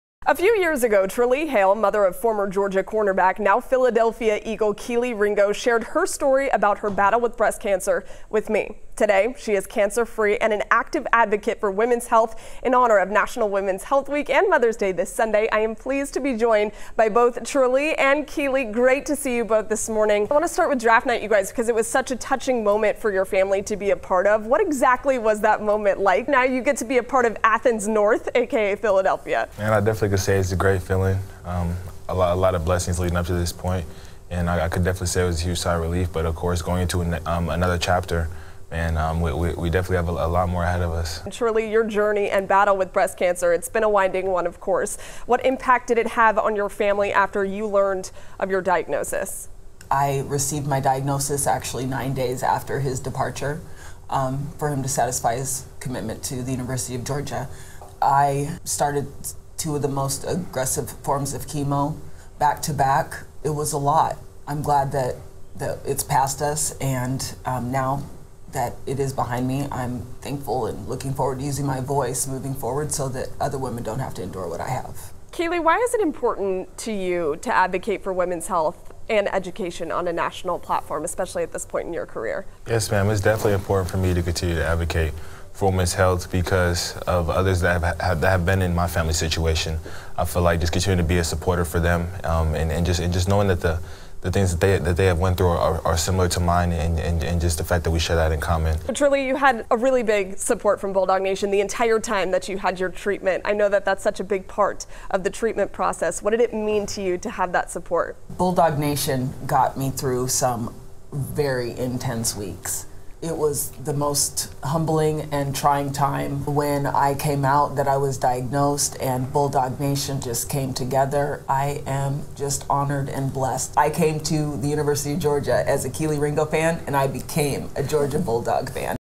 Former Georgia Bulldog advocating for women's health alongside his mom, a breast cancer survivor
Newly drafted Philadelphia Eagle Kelee Ringo and his mother are using their platform to educate women about breast cancer and personal health.